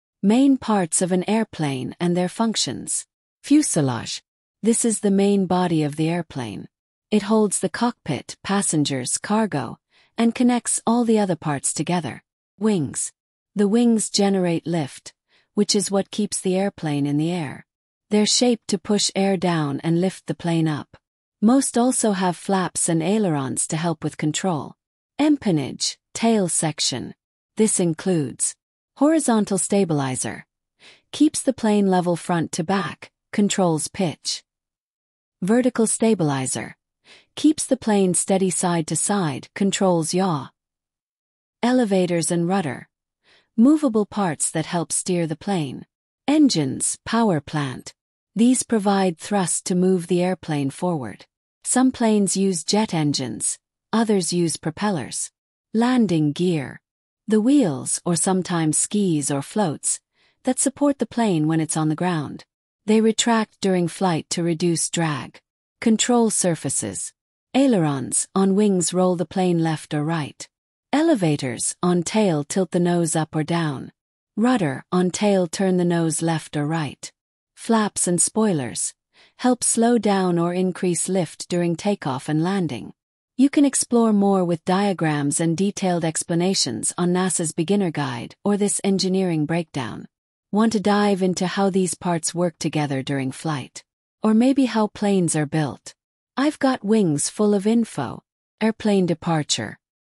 Airplane Anatomy: Main Parts and Their Functions Explained
This video explores the essential components of an airplane and how they work together to achieve flight. The video covers:
Fuselage: The central body that holds passengers and crew.
Wings: Generate lift to keep the plane airborne.
Tail (Empennage): Provides stability and control.
Engines (Powerplant): Produce the thrust needed for forward motion.
Landing Gear: Supports the aircraft on the ground.
Learn the basics of aircraft design and function!